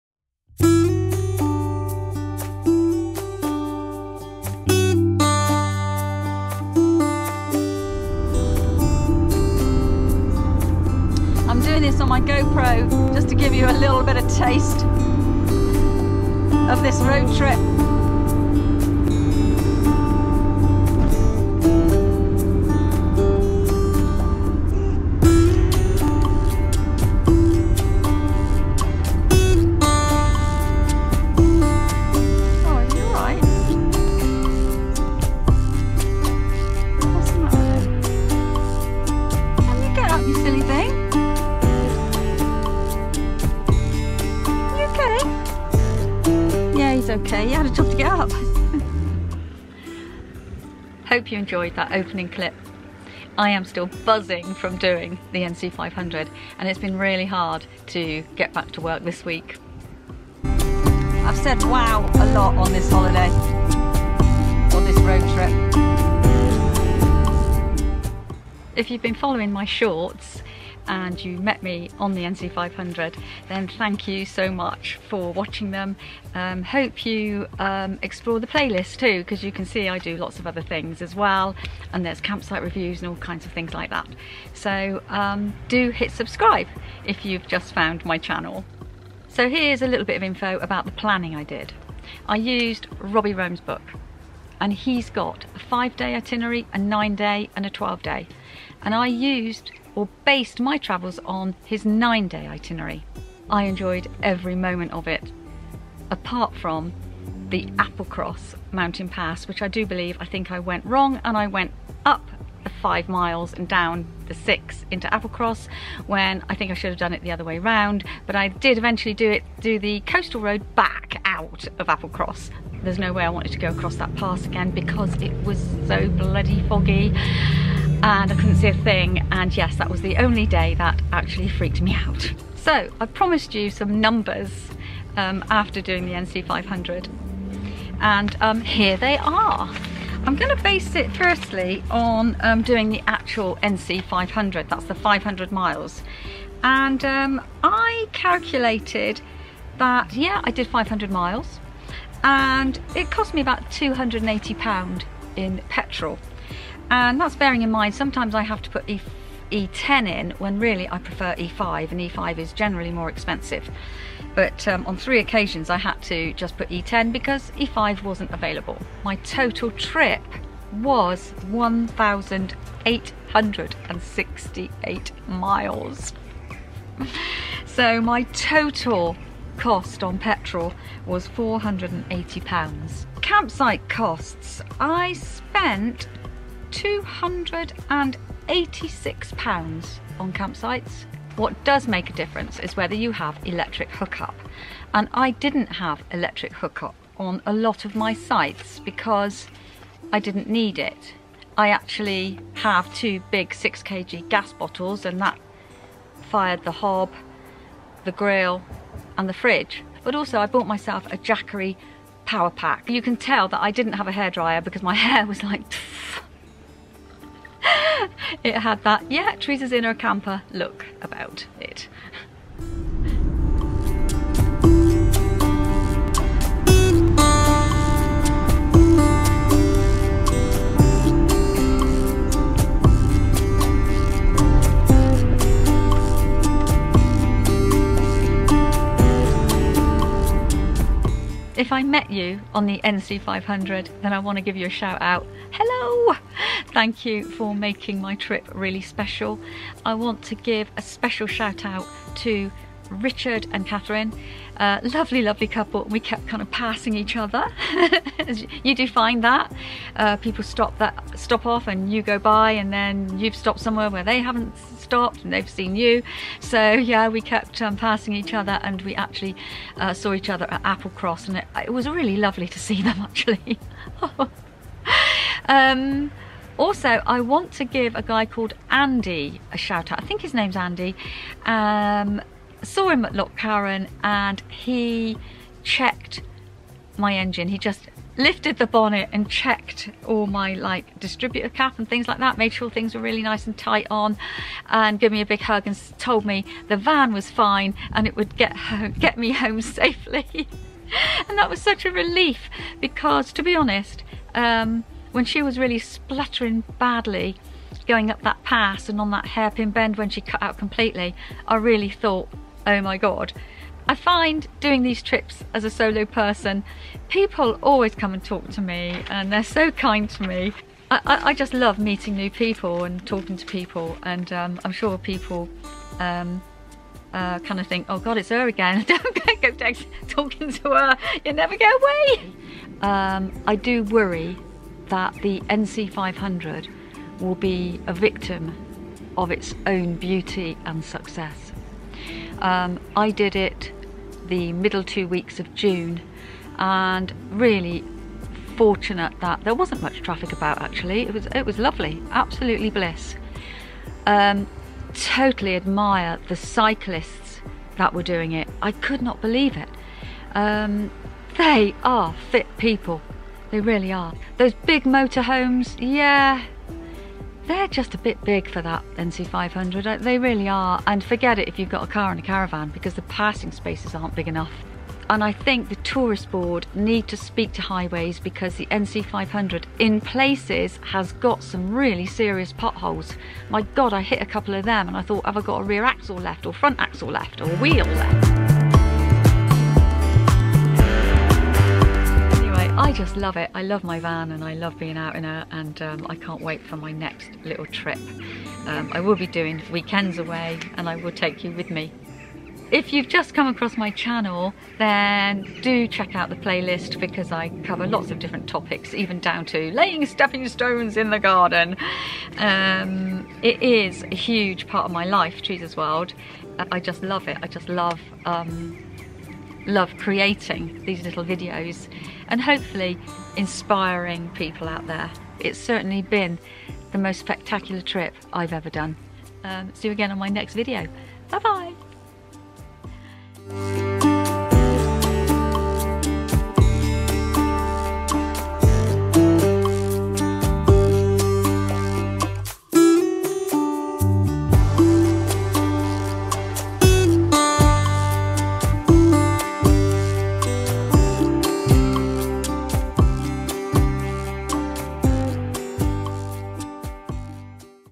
NC500 Roadtrip footage and general info, fuel, campsites mileage
A concluding video about my NC500 road trip, covers the numbers, for those who asked for them, ie mileage, fuel, costs etc. But hey, WOW! What a spectacular trip - has to be the best road trip I've ever done. Thank you to Robbie Roams and his great book... full of useful info, so very helpful. Thank you to the people I met, who helped make my trip a memorable one.